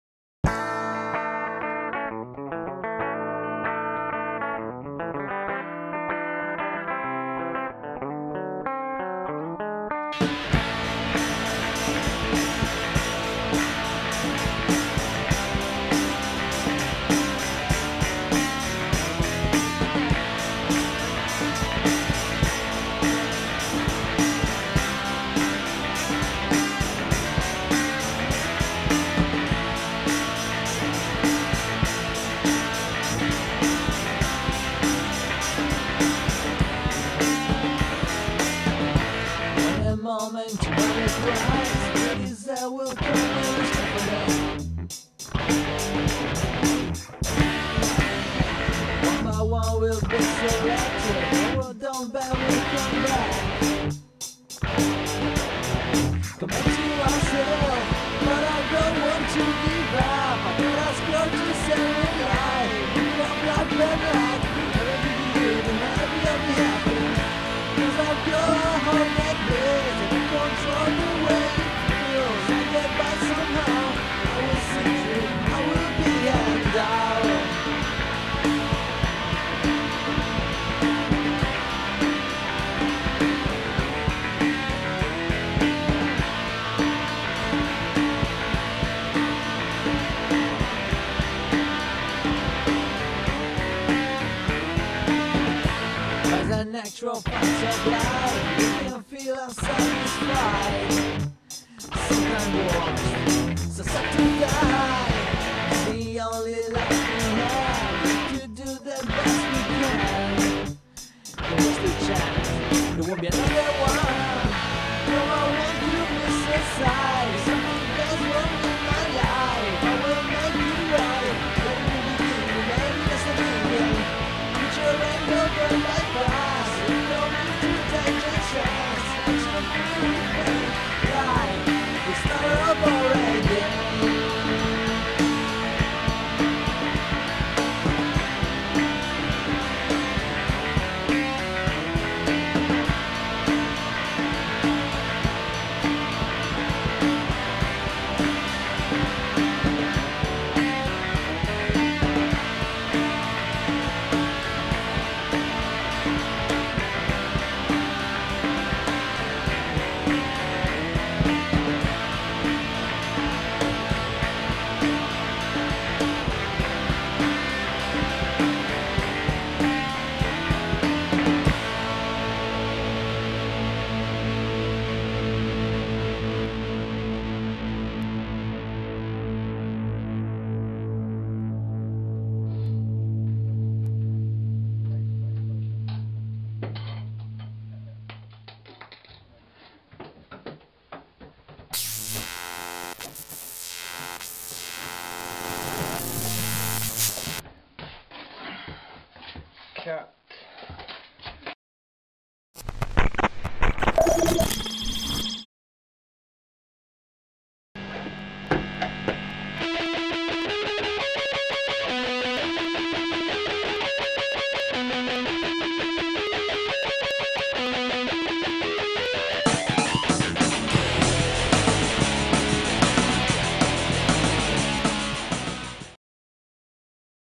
DIY Music Video (Clones Effect) "REMASTERED"
This is NOT a band ! It's me recording the song and then filming myself for the video.
It's a mush-up of time-lapse and cloning effect.
The synchronization starts at 1:31

Mecis-AKA-Matches